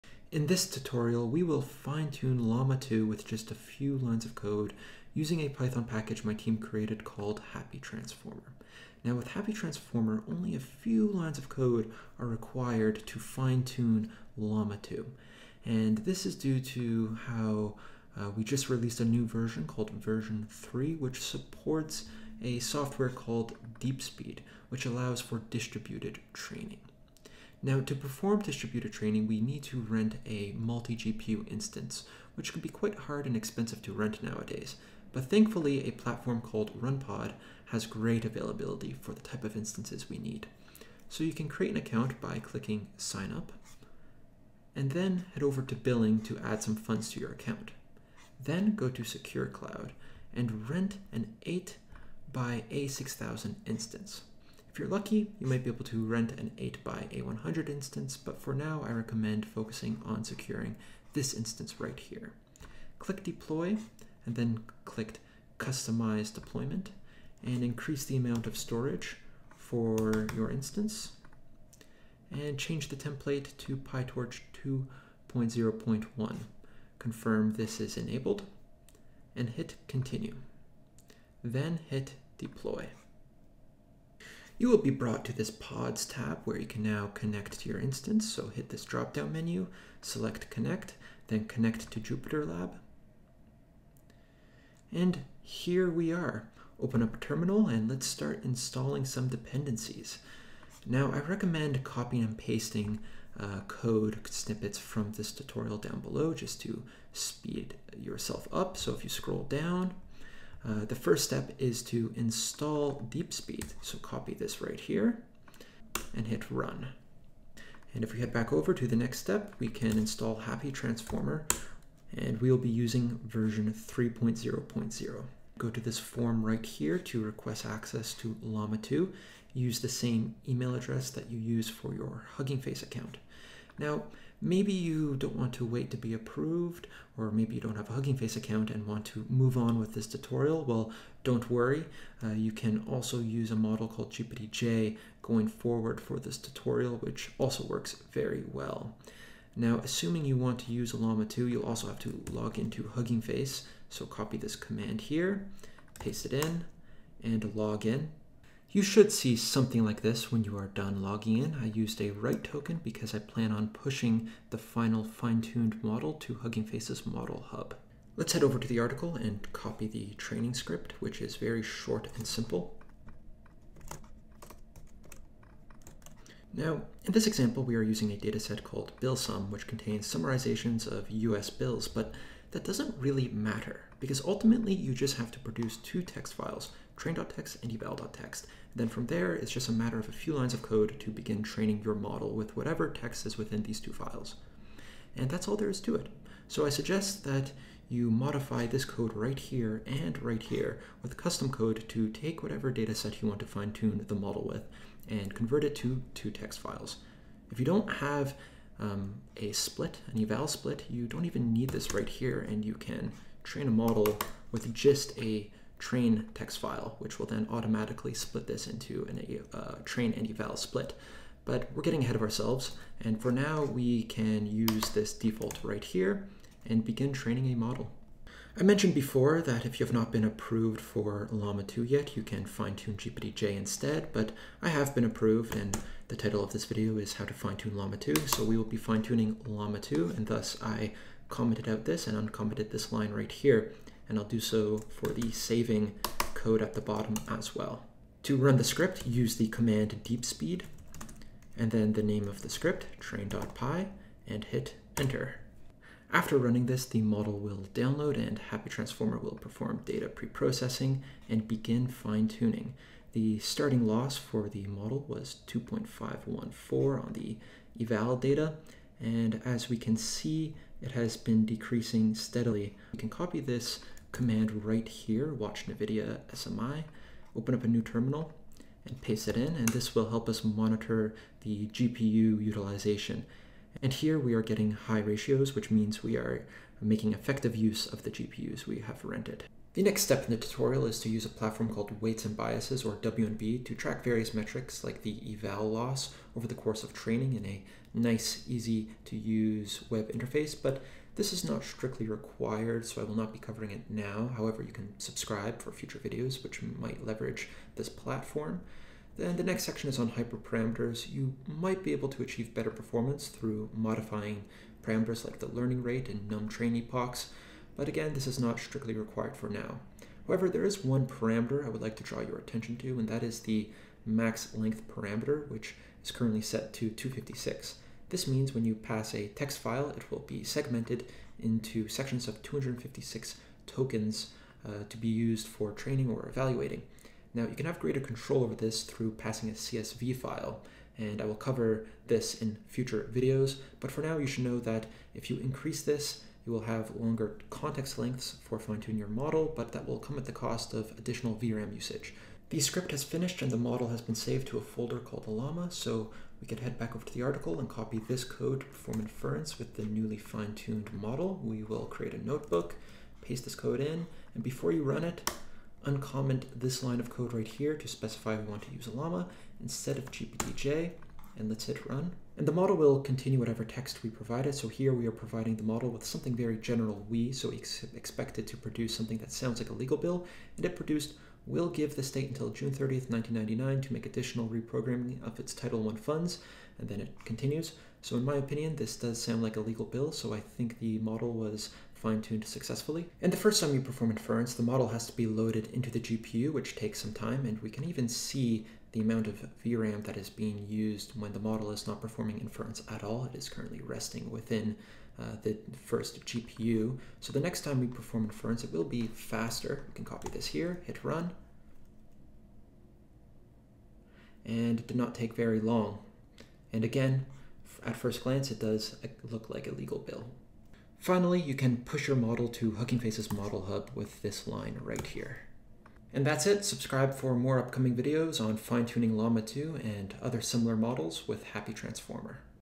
Fine-Tune Llama-2 Easily With Happy Transformer and DeepSpeed
Llama-2 made easy. Learn how to fine-tune and perform inference with Llama-2 using only a few lines of Python code. We'll use a Python package called Happy Transformer that leverages DeepSpeed to train the model.

0:00 – Intro
0:27 –RunPod 8xA6000 setup
1:35 – Installation
2:04  – Llama-2 request
2:30 — Hugging Face login
2:47 – Training script
4:32 – Monitor GPU utilization
4:48 – WandB
5:08 – Hyperparamaters
5:54 – Inference
7:00 – Upload to Hugging Face
7:07 – Conclusion

Vennify Inc.